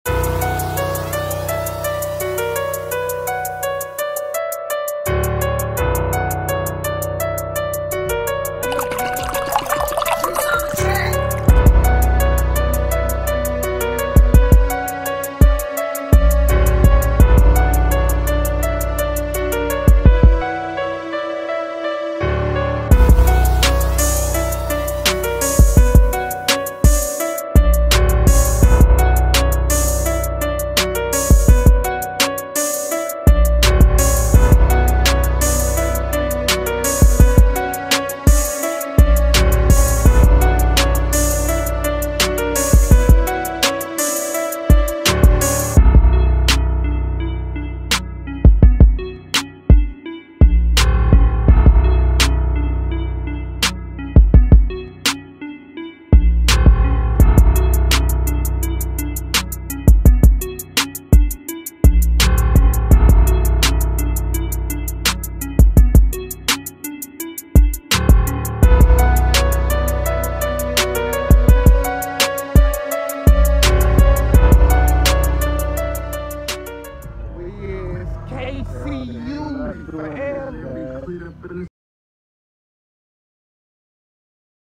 Kennel Clubs United (K.C.U) American Bully Promo
The Kennel Clubs United is not your typical club. They are a family dedicated to breeding the best American bullies. Their goal is to build a club known for its high quality, standards, and strong sense of community.

In this video, you see a live demonstration of one of their family gatherings. If interested, the club is always looking for new members who want to breed the best and be apart of a bigger family.